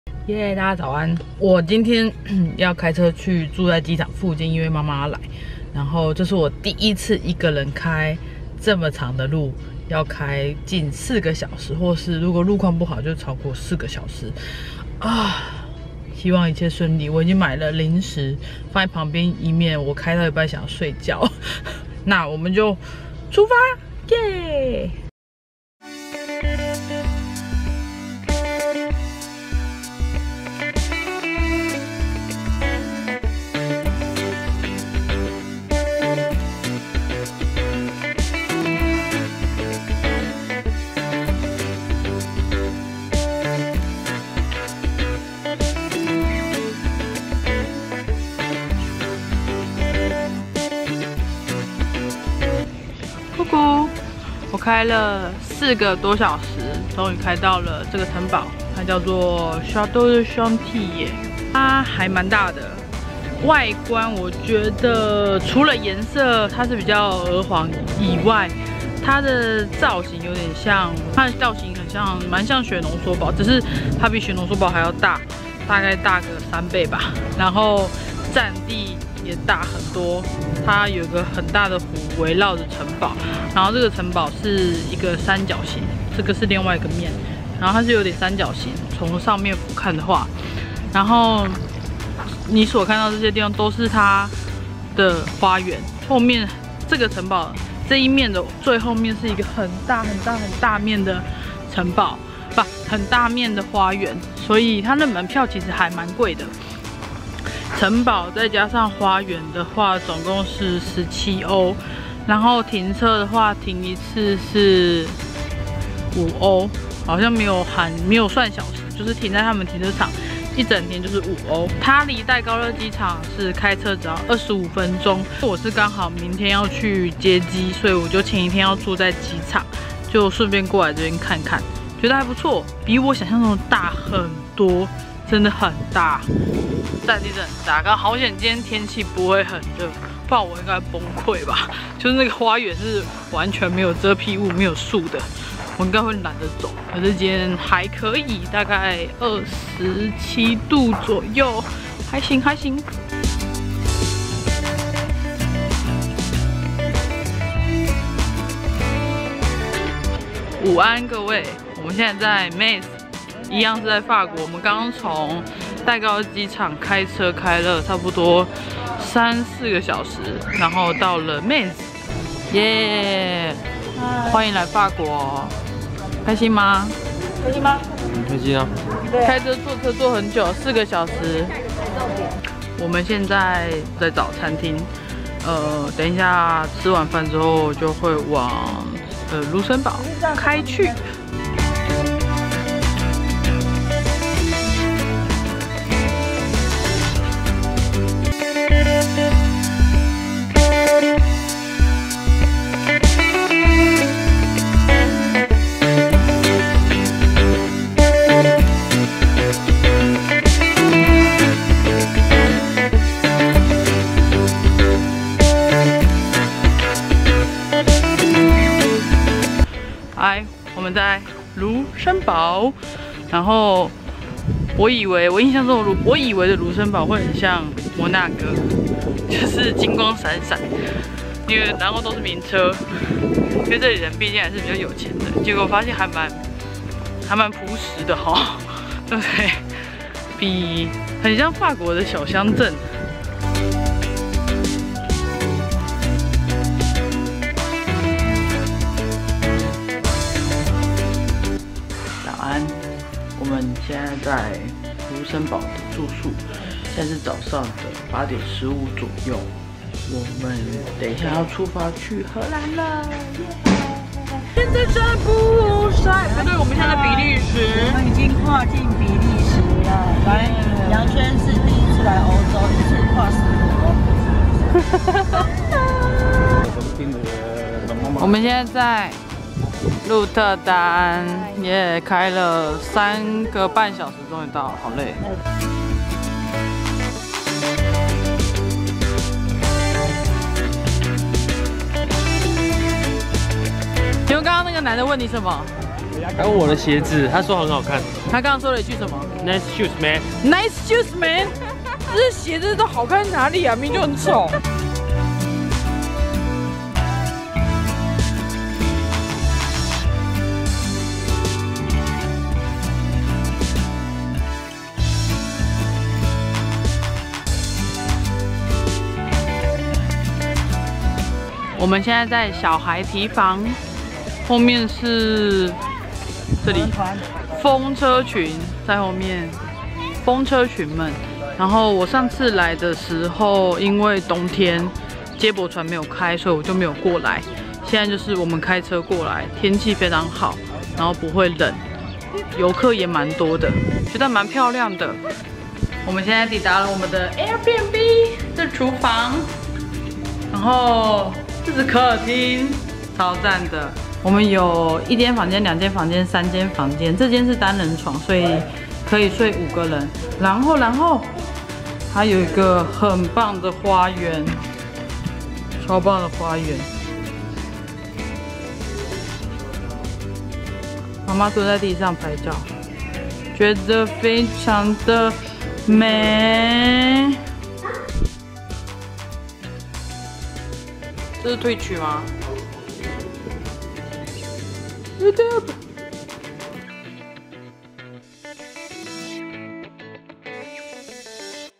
2019弟弟歐洲初上陸，帶媽媽遊比利時.荷蘭.盧森堡
AIRBNB

追蹤IG看看我的法國生活
歡迎加入我的法國小生活(連結在下

影片使用相機為：Canon G7 X Mark II